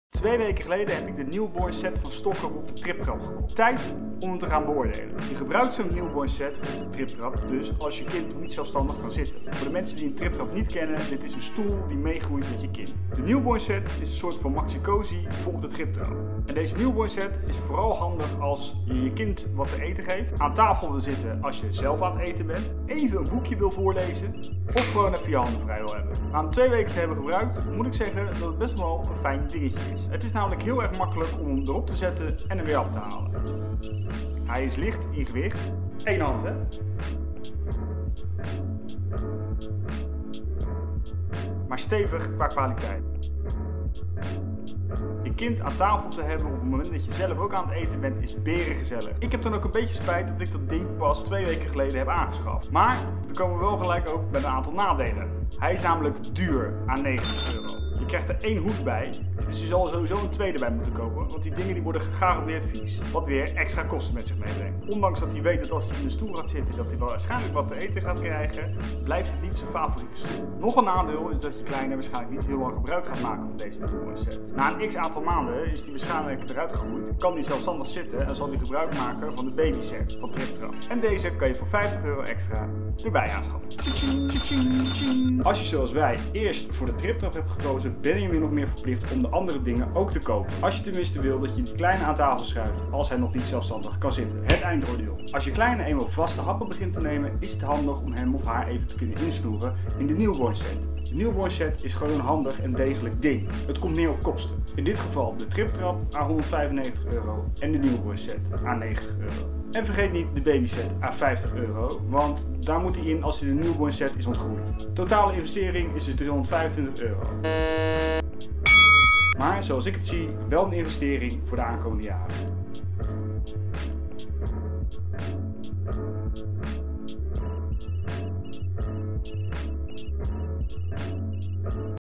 Review newborn set Stokke Tripp Trapp Papa's perceptie Afl.11
In deze video review ik de newbornset voor op de Tripp Trapp van Stokke. Best een handig dingetje maar komt met wat nadelen.

Als aankomende vader heb je geen idee wat je te wachten staat. Je hoort de verhalen maar je weet gewoon niet wat je te wachten staat.

In deze video's deel ik mijn ervaring als beginnend vader.